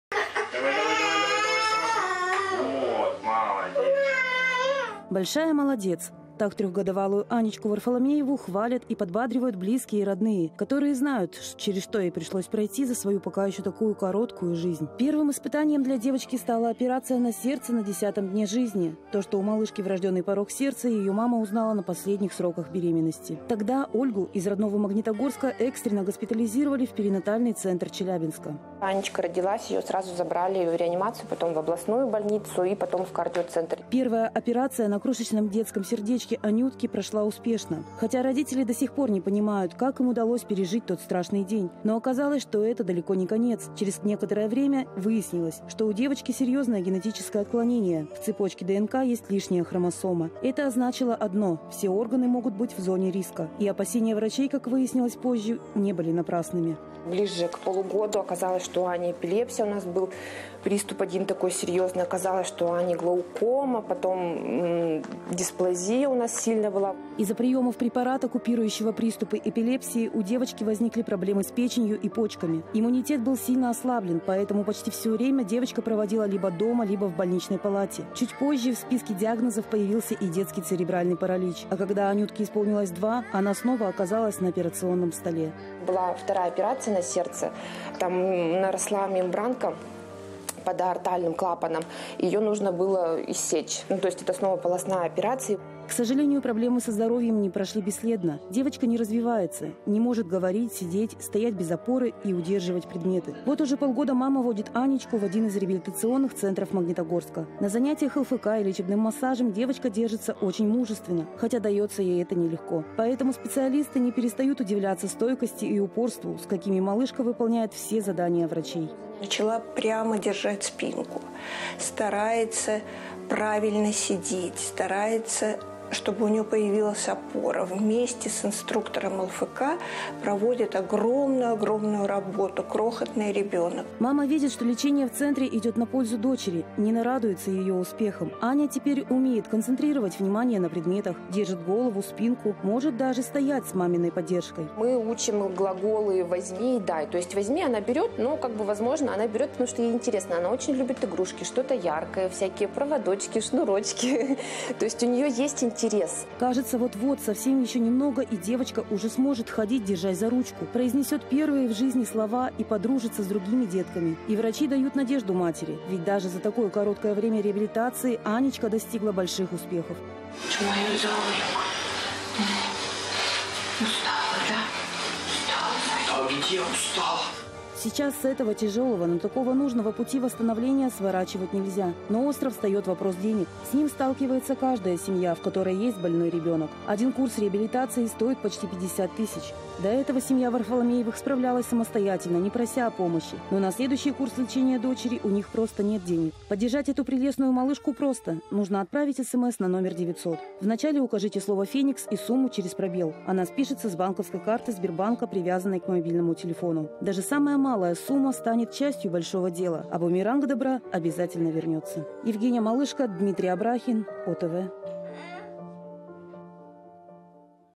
Добрые среды — Аня Варфаломеева
«Большая молодец!» — так трехгодовалую Анечку Варфаломееву хвалят и подбадривают близкие и родные, которые знают, через что ей пришлось пройти за свою пока еще такую короткую жизнь. Первым испытанием для девочки стала операция на сердце на десятый день жизни. То, что у малышки врожденный порок сердца, ее мама узнала на последних сроках беременности.

Тогда Ольгу из родного Магнитогорска экстренно госпитализировали в перинатальный центр Челябинска. Первая операция на крошечном детском сердечке Анютки прошла успешно. Хотя родители до сих пор не понимают, как им удалось пережить тот страшный день.